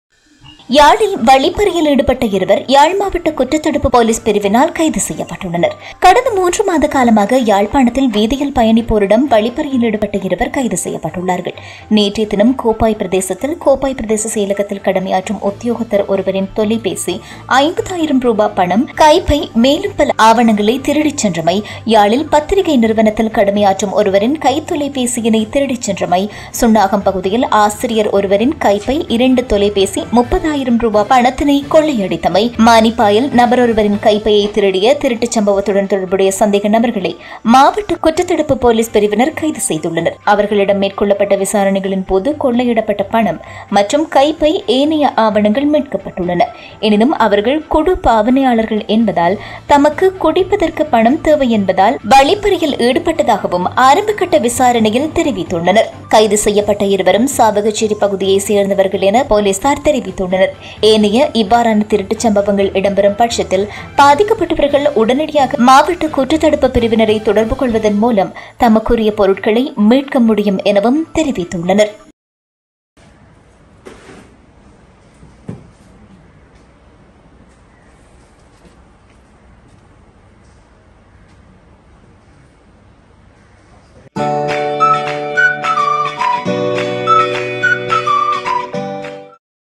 யாழில் குடிப்பதற்காக வழிப்பறியில் ஈடுபட்ட இரு ஆசாமிகள் சிக்கினர் - வீடியோ
எங்கள் இணையத்தளத்தையும் பார்வையிடலாம் 👀

மேலும் செய்திகள்